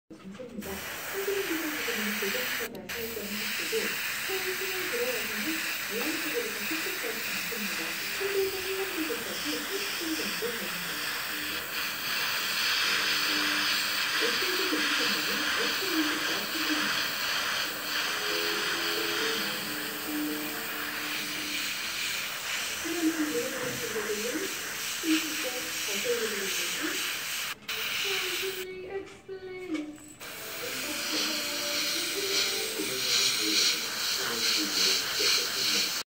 튜닝타임즈 프라모델 강좌 영등포팀 이종원님의 Aoshima 아오시마 1/48 Full Metal Panic! 풀메탈패닉 M9 Gernsback 건즈백 마스킹 색칠
신세계백화점 신세계아카데미 영등포점 문화센터 프라모델 에어브러시 수업 수강생 이종원님이 지난번에 명암 색칠을 해줬던 풀메탈패닉 건즈백에 기본색을 올렸는데요, 부품 분할이 덜 되어 있어서 일일이 마스킹을 하고 진한 색상을 칠해가고 있습니다^^